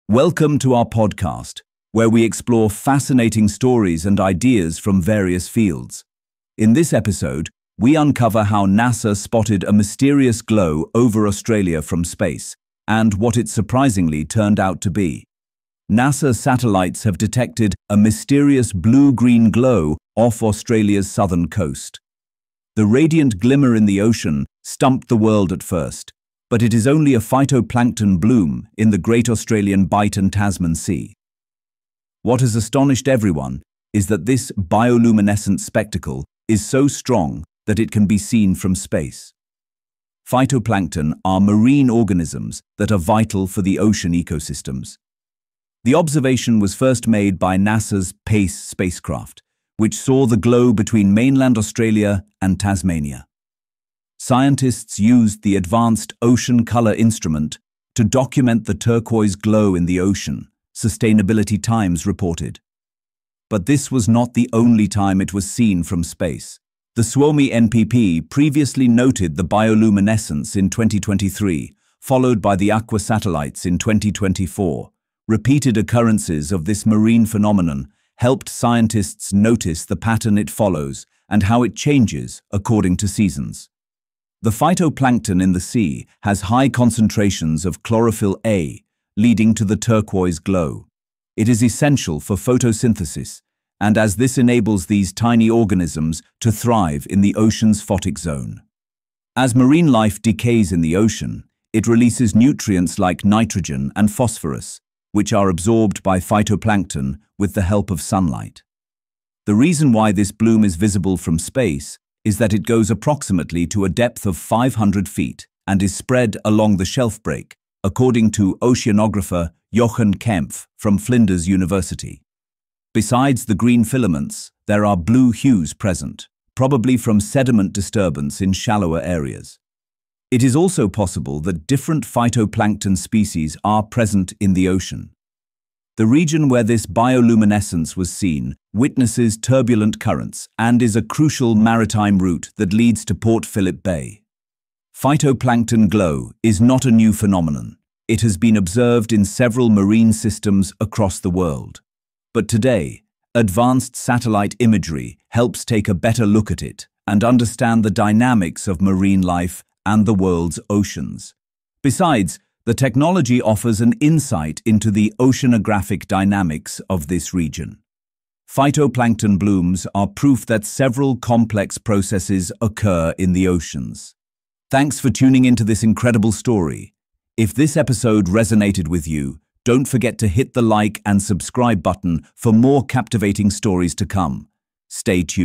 NASA Detects Glowing Ocean Phenomenon Off Australia’s Coast | WION Podcast
NASA satellites have captured a spectacular turquoise glow off Australia's southern coast, caused by a massive phytoplankton bloom in the Great Australian Bight and Tasman Sea. This bioluminescent phenomenon, visible from space, highlights the role of phytoplankton in ocean ecosystems. Detected by the PACE spacecraft and other satellites, the bloom is fueled by chlorophyll a and nutrient-rich waters. It spans hundreds of miles and reveals the dynamic processes happening beneath the ocean's surface. Scientists are using this data to better understand marine life and oceanographic patterns.